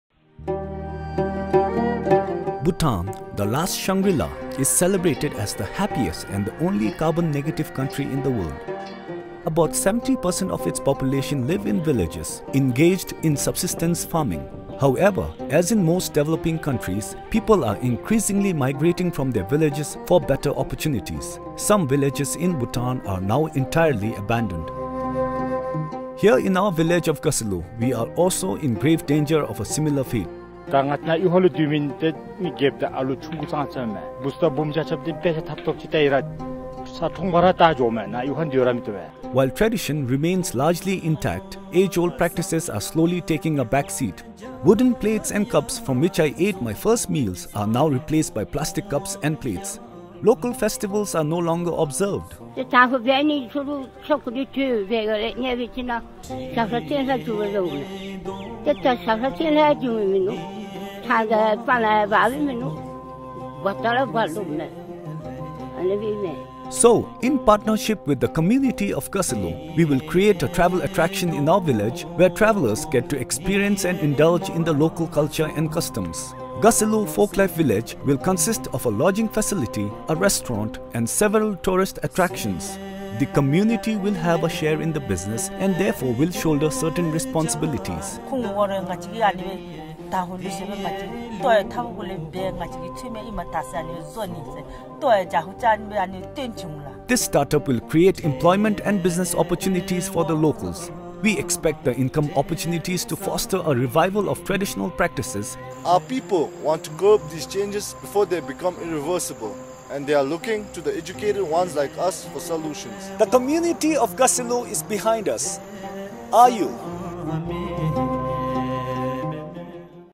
Gaselo Folklife Village
Gaselo Folklife Village - A Social Venture in Community Based Tourism.

Bhutan is one of the top exotic travel destinations in the world but the variety of cultural and experiential sights and products in Punakha/Wangdue region are limited, although a popular destination. Tourists and tour operators want more.

Gaselo Folklife Village, a social enterprise, aims to cater to this unmet demand. It is backed by the projected strength of the market and visitor trends while bringing the benefits of tourism to the grassroots.

In partnership with the community of Gaselo, Gaselo Folklife Village will create lodging facilities and travel attractions in a village in Gaselo - where travelers get to experience and indulge in the local culture and customs.

This venture will therefore curb the increasing rural urban migration by creating employment and business opportunity to the locals of Gaselo. It is also expected that the income opportunities will foster a revival of traditional practices, including the age-old organic farming method.